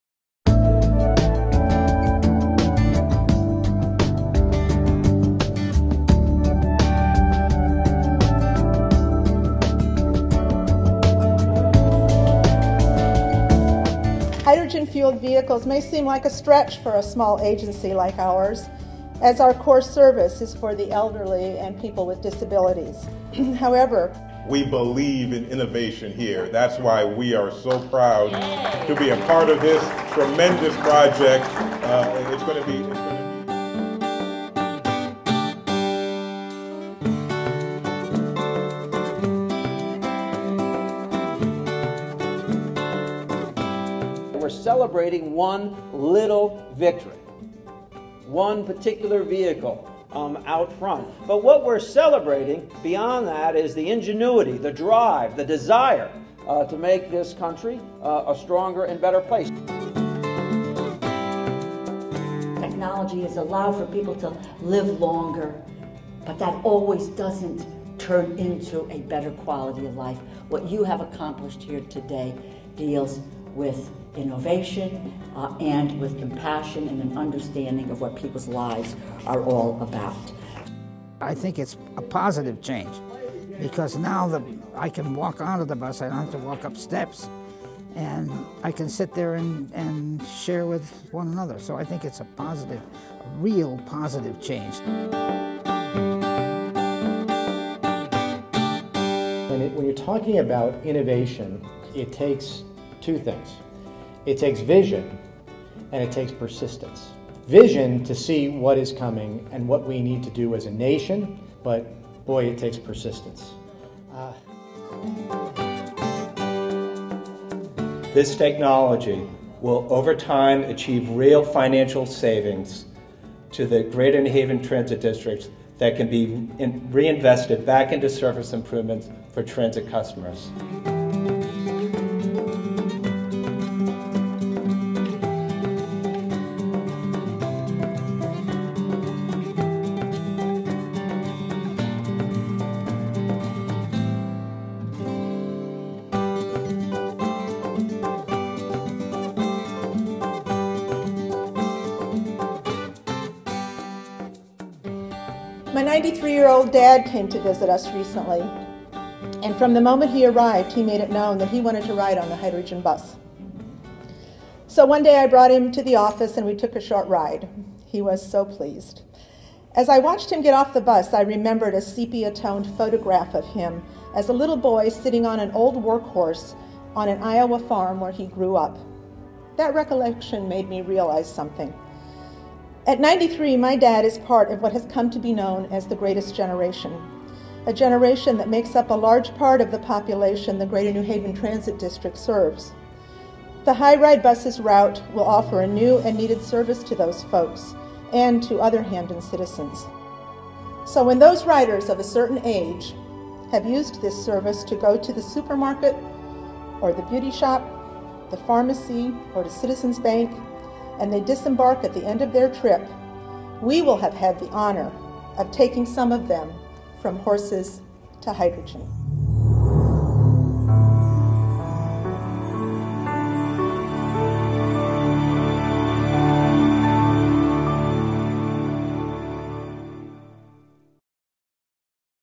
Green Revolution! Hydrogen Power Comes to Connecticut
Hy-Ride, the new hydrogen-powered bus, is now in service, thanks to the cooperative efforts of the Greater New Haven Transit District and the State of Connecticut.